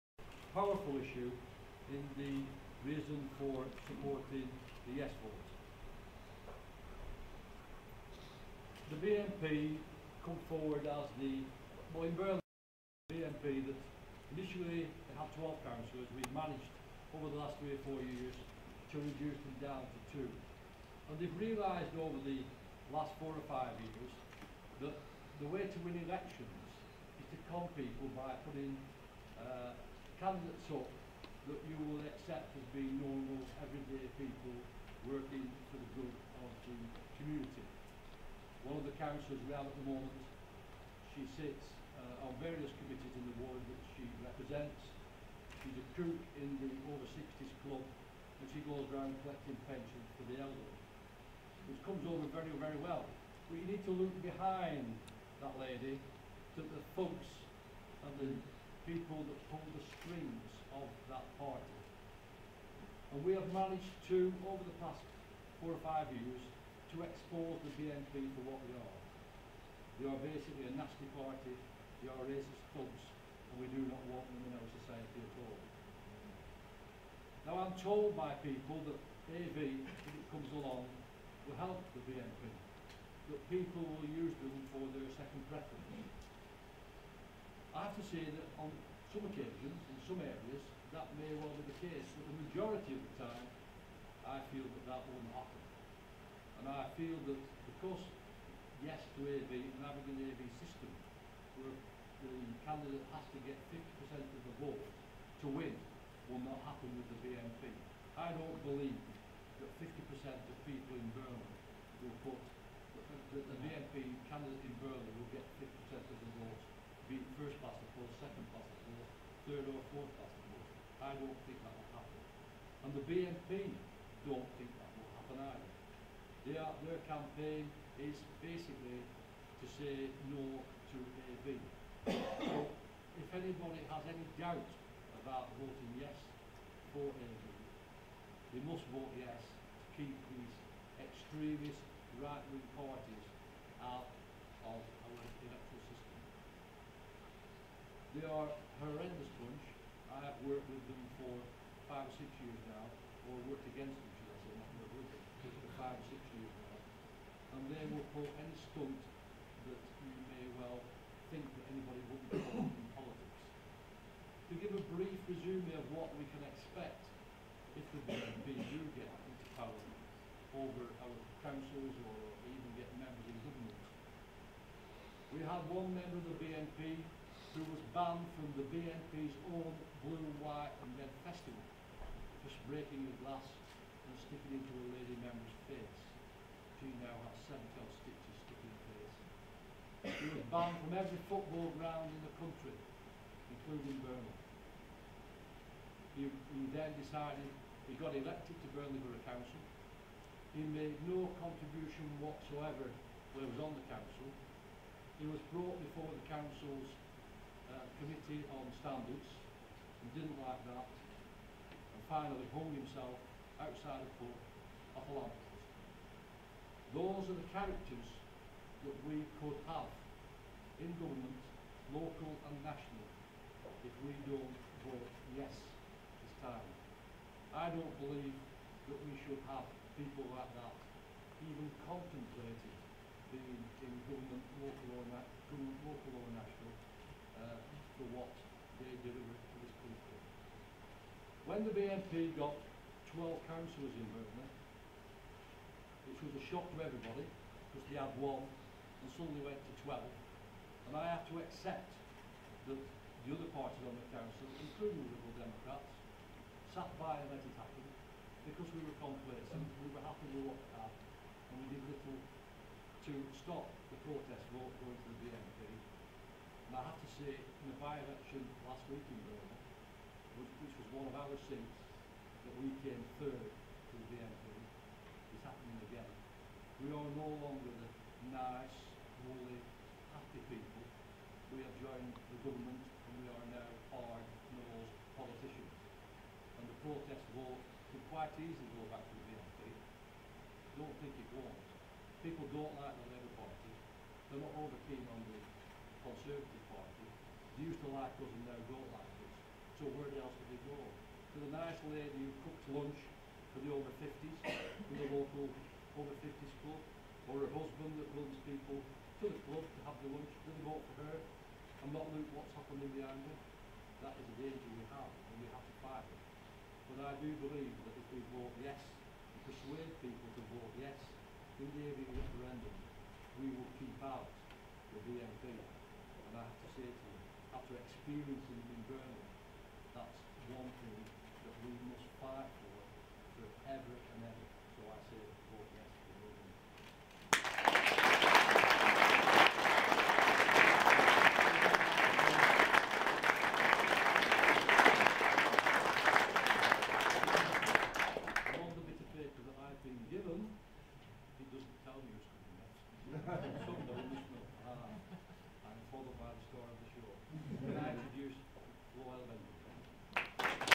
gordon birtwistle yes
Gordon Birtwistle MP says yes to AV